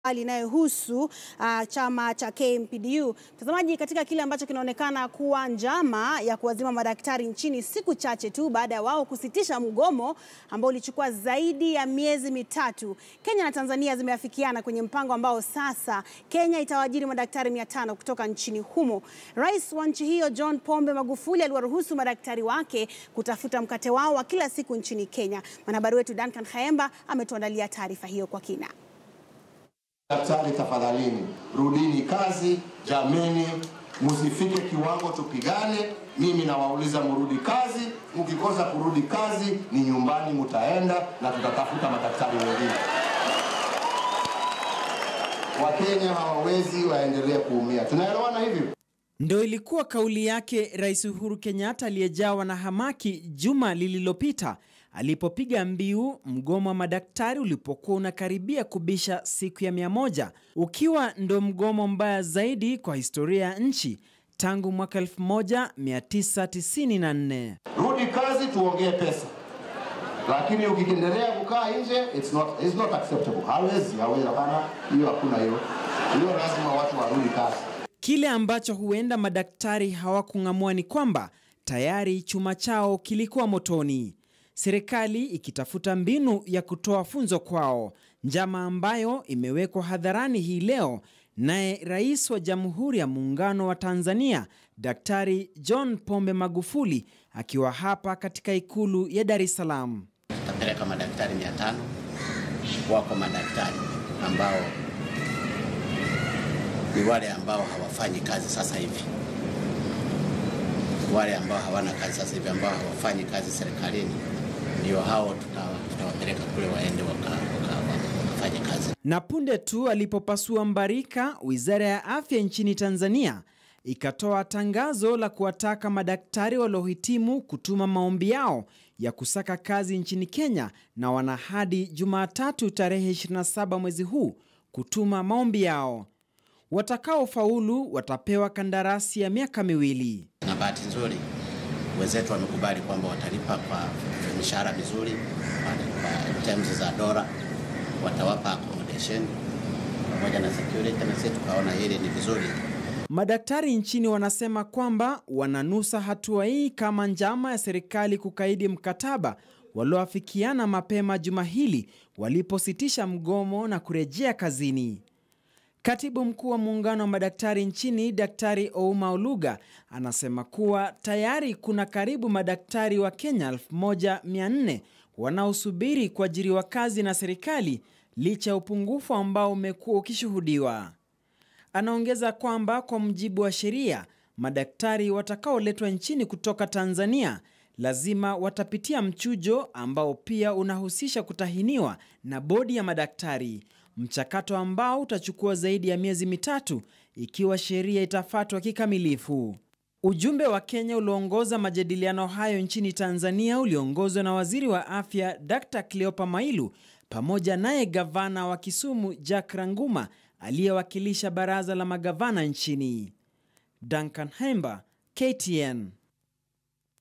Rais wa Tanzania-John Pombe Magufuli awaruhusu madkatari wake kutafuta mkate wao wa kila siku nchini
Katika kile kinacho onekana kuwa njama ya kuwazima madaktari nchini siku chache tu baada ya wao kusitisha mgomo uliochukua zaidi ya  miezi mitatu, Kenya na Tanzania zimefikiana kwenye mpango ambao sasa Kenya itawaajiri madaktari 500 kutoka nchini humo. Rais wa nchi hiyo John Pombe Magufuli aliwaruhusu madkatari wake kutafuta mkate wao wa kila siku nchini Kenya.